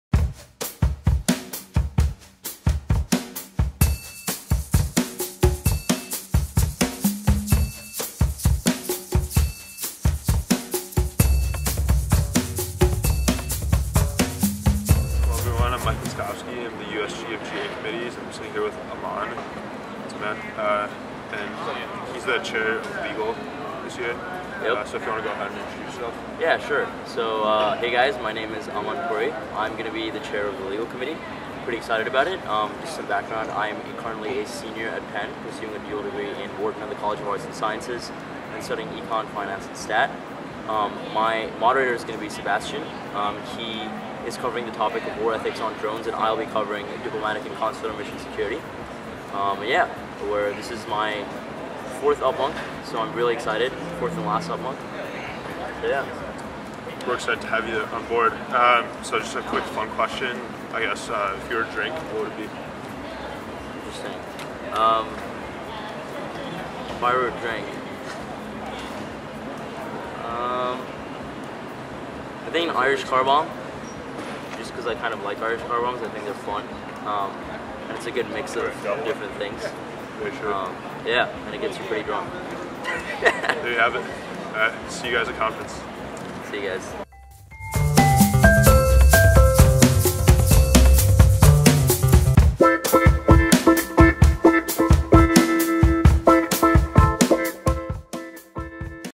Meet the Dais- Legal
Committee Information
Title: Sixth Committee: Legal

Organ: General Assembly

Chairs: Aman Puri and Sebastian Negron-Reichard

Under-Secretary-General: Michael Mintskovsky

Topic A: Diplomatic and Consular Mission Security

Topic A Summary:

Particularly as of late, threats to diplomatic missions have become a growing worldwide concern. The Middle East in particular is a dangerous place for embassies and diplomatic missions. In early 2015, Yemen marked the third Middle Eastern country from which U.S. diplomats have withdrawn, following Lybia and Syria. Even more recently, missions in Saudi Arabia have faced security concerns, and have closed down for periods of time in response. The security of foreign missions is inevitably tied to legal matters. Specifically, there is gray area when it comes to the responsibilities of a host country versus those of an embassy. According to the Vienna Convention on Diplomatic Relations, it is the duty of the receiving nation to protect the mission from any intrusion or damage and prevent and disturbance of peace. However, recent events have planted seeds of doubt of the ability of host nations to protect embassies from outside attacks by rogue groups. Although the issue of diplomatic mission security is not without U.N. precedent, those guidelines have proven to do little to stop threats to embassies. Vast political instability in the wake of the Arab Spring, cultural clashes, and other forces have rendered diplomatic and consular mission security a controversial, complex, and prevalent issue.

Topic B: War Ethics of Drones

Topic B Summary:

The exponential growth of technological advances is a double-edged sword. Although serving countless civilian applications, technology has historically been intertwined with war tactics. In the past few decades, the development and use of drones (unmanned aerial vehicles) has become a huge part of the war-related intelligence revolution. In the early 2000s, the U.S. pioneered the use of drones as military weapons (as opposed to just tools for reconnaissance). Drones have effectively evolved into a means of carrying out attacks without risk of directly losing lives, and therefore have transformed the very notion of war. The unmanned drones give an impersonal aspect to war, detaching combat from humanity. When drone strikes define legal strike zones, there is gray area as to what is considered a "legal killing." The traditional notions of battlegrounds tend to fade away. "Drone War" attacks have been responsible for several thousand casualties just in the last five years. Therefore, we must call into question the ethics of drone strikes. Is there a way to use them in an ethical way? Should there be restrictions of research & development as well as production of drones?